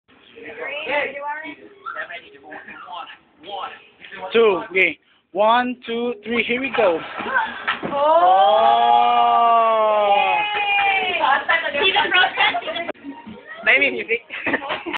warrens back flip.
this time, he kinda got the back flip with the major of course w/ jeff dan and gian his idols. hehehehe. congrats war...